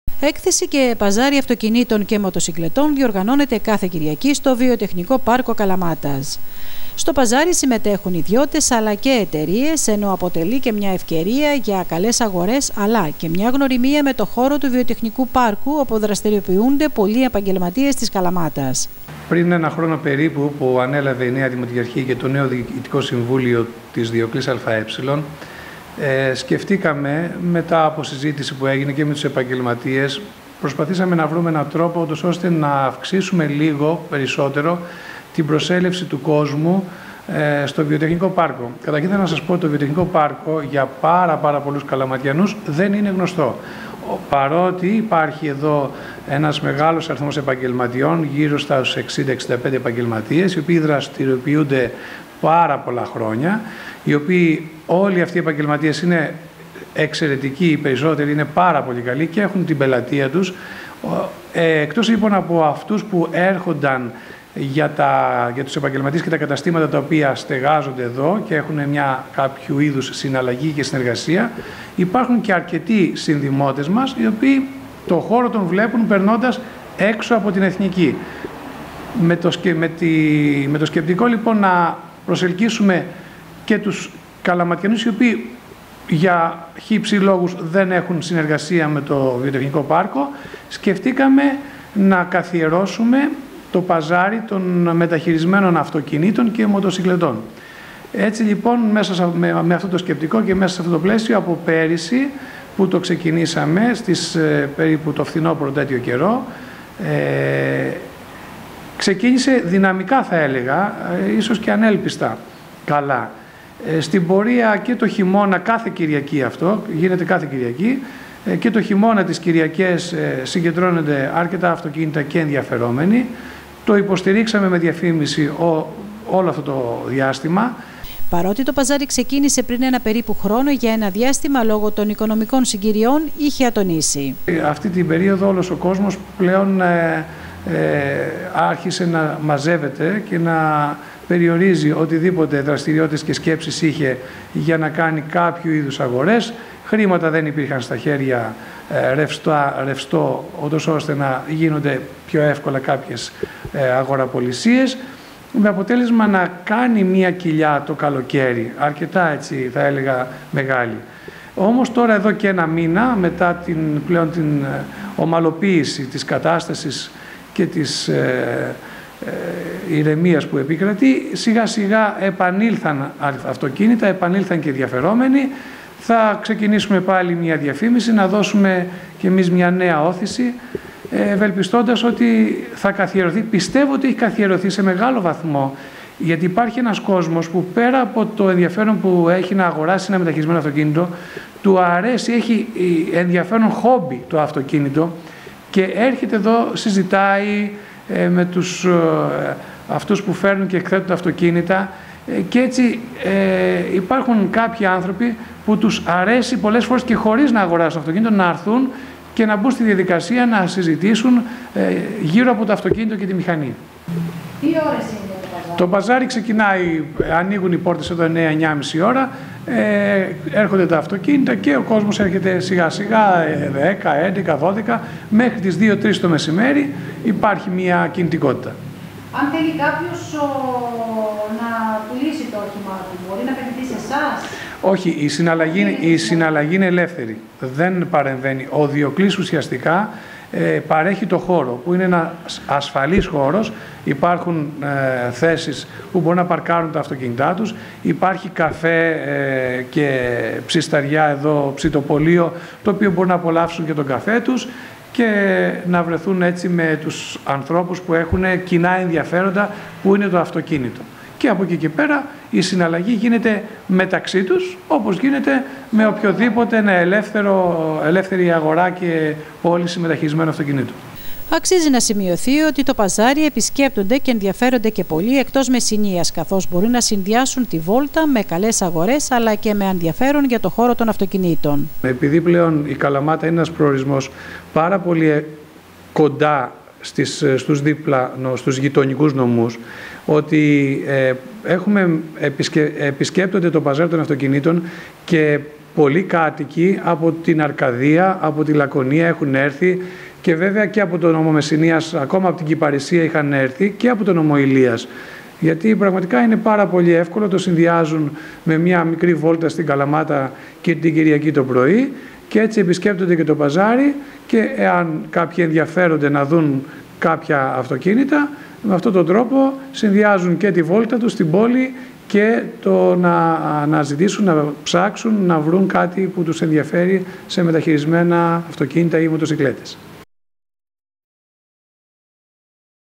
EKTHESH AYTOKINHTON BIOPA - PAZARI DIOKLHS 4 11
Συνεχίζεται το παζάρι αυτοκινήτων και μοτοσυκλετών στο βιοτεχνικό πάρκο Καλαμάτας το οπόίο έχει ξεκινήσει περίπου ένα χρόνο. Για ένα διάστημα λόγω των οικονομικών συγκυριών το παζάρι είχε ατονήσει αλλά η δημοτική επιχείριση Διοκλής προσπαθεί τώρα να αναθερμάνει το ενδιαφέρον των πολιτών . Έτσι κάθε Κυριακή απο τις 9 το πρωί μέχρι αργά το απόγευμα όποιος ενδιαφέρεται μπορεί να επισκεφθεί το χώρο.